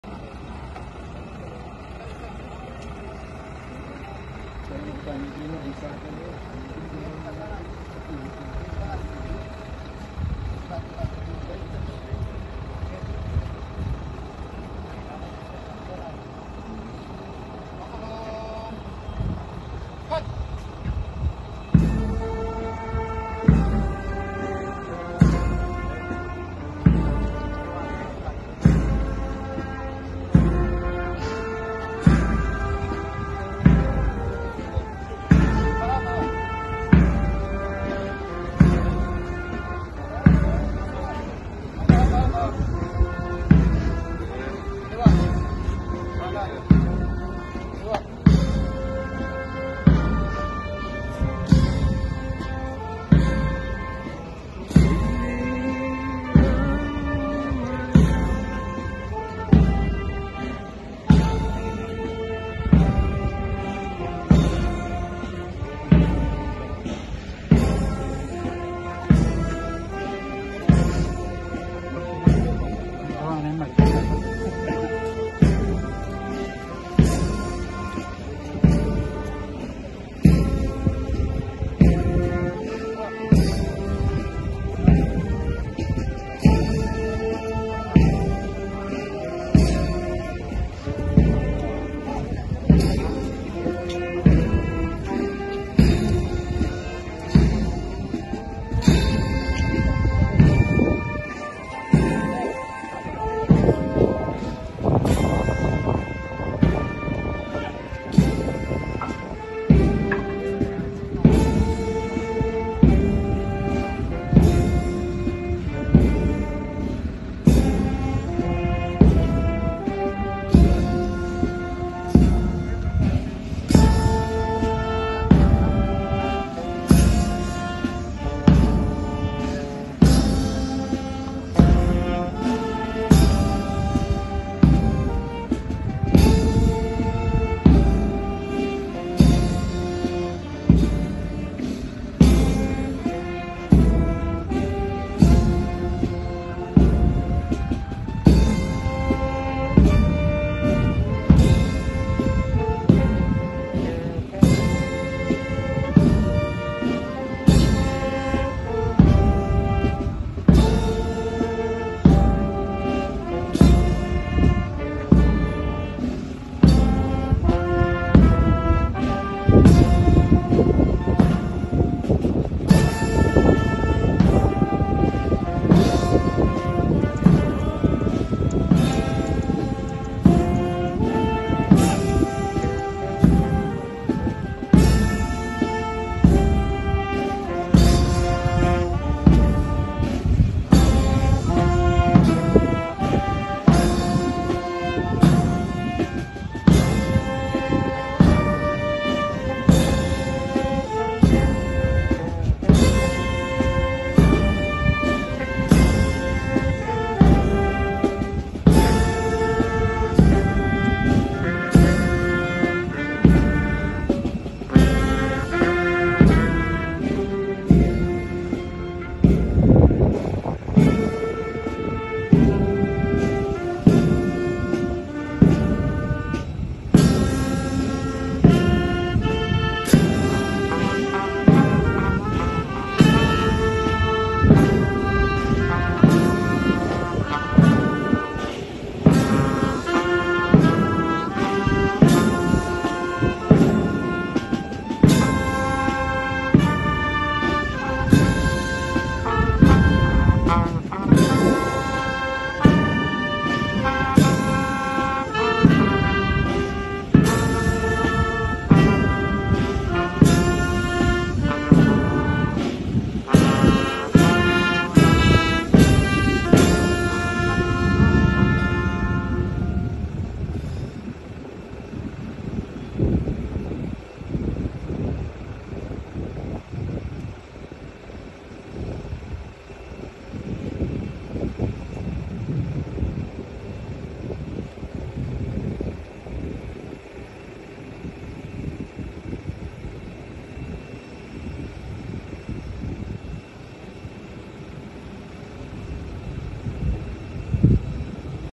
PNP renders Arrival Honors to Ex-ICPO Director and PCSO Board Secretary Atty. Wesley Barayuga
The Philippine National Police renders arrival honors to the remains of former Iloilo City police director and now Philippine Charity Sweepstakes Office board secretary Atty. Wesley Barayuga.

🎥 Joey Contreras